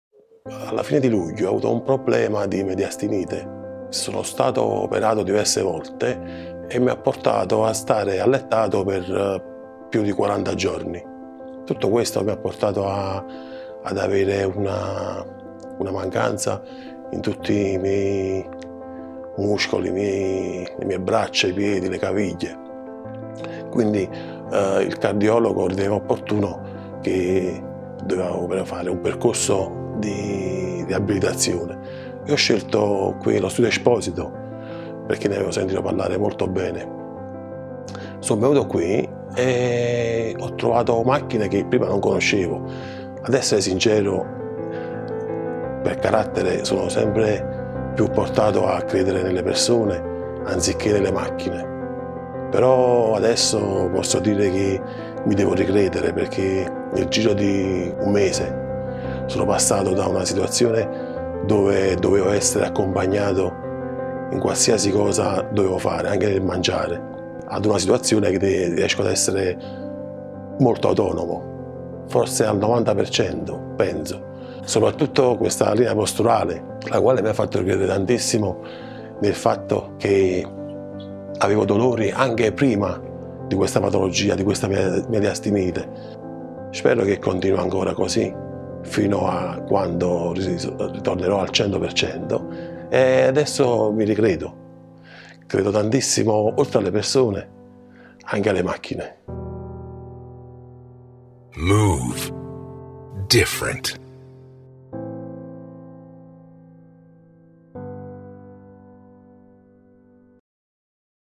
TecnoBodyTrueStories
Every day we do research and develop our systems thinking of the users of the technology we use: the patients of the centers that choose us.
This testimony, a true story of a difficult path, gives us back the satisfaction for our daily commitment, which together with the skilled hands of the partners who embrace the TecnoBody philosophy, contributes to the effectiveness of motor recovery paths.

Move Different!